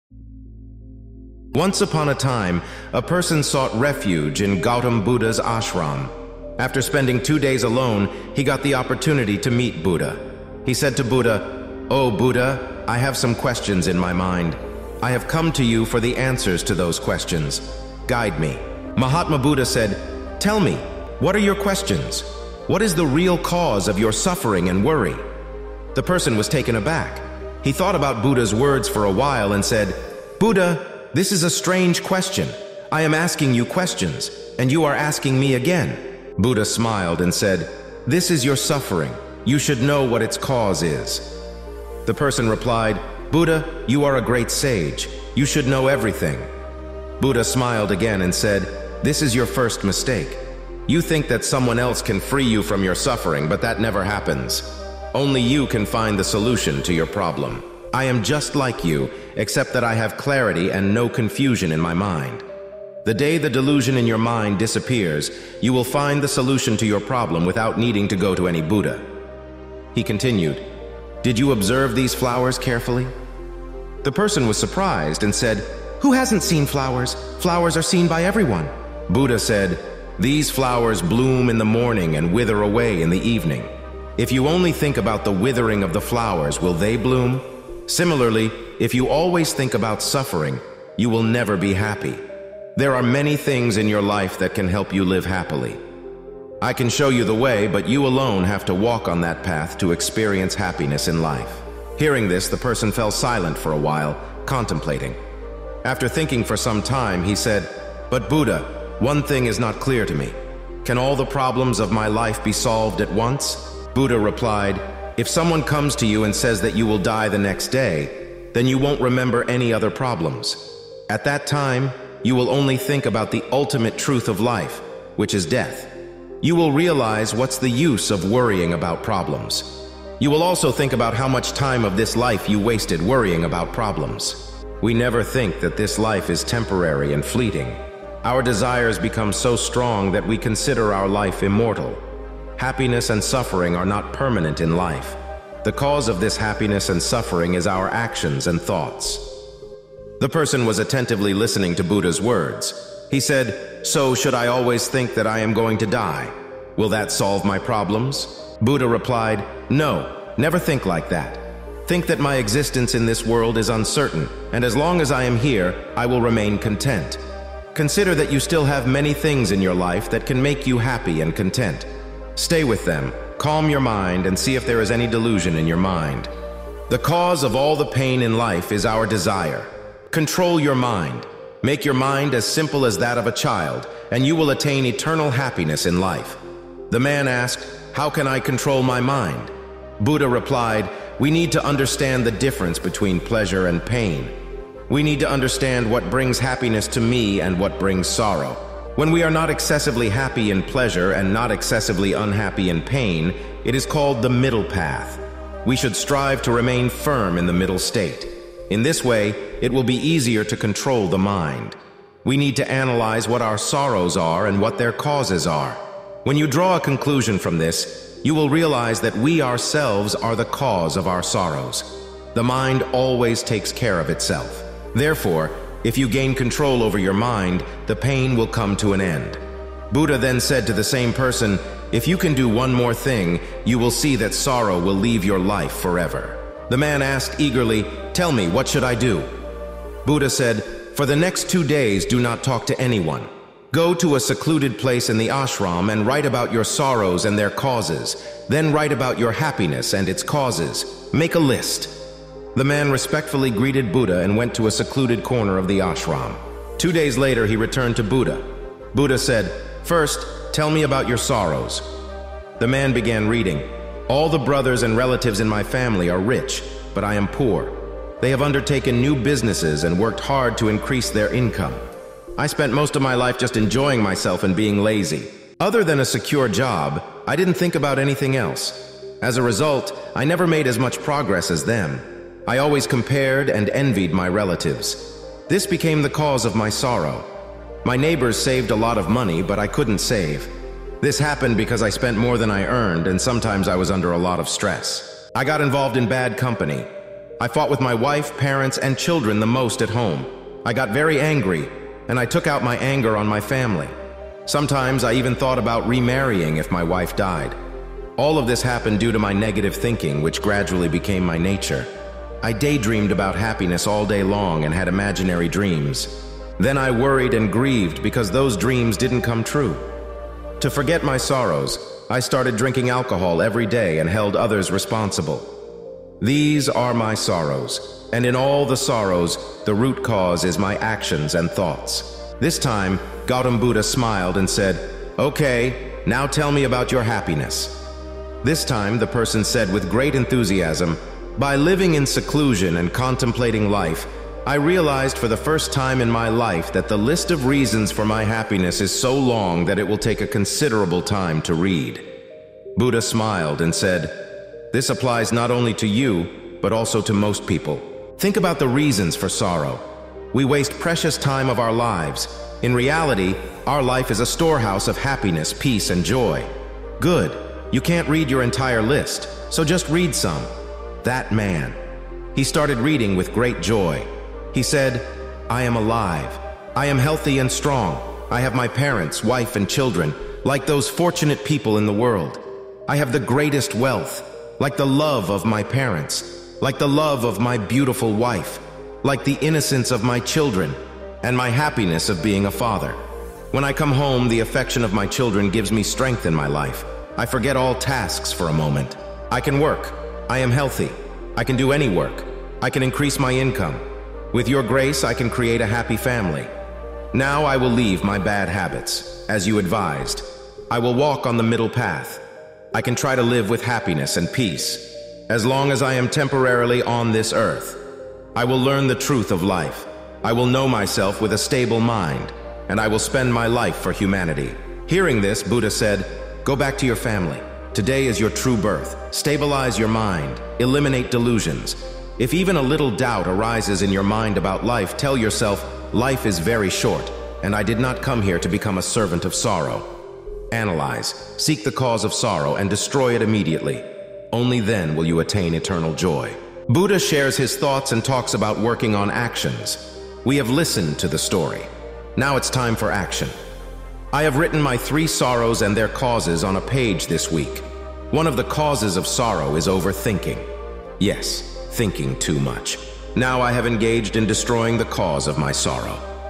Learn How to End Sadness and Find Happiness - A Simple Zen Story
Discover powerful strategies to overcome sadness and embrace a happier life. Explore effective techniques, tips, and guidance to conquer sadness once and for all. Let's chase away the clouds of gloom and let the sunshine in! 🌞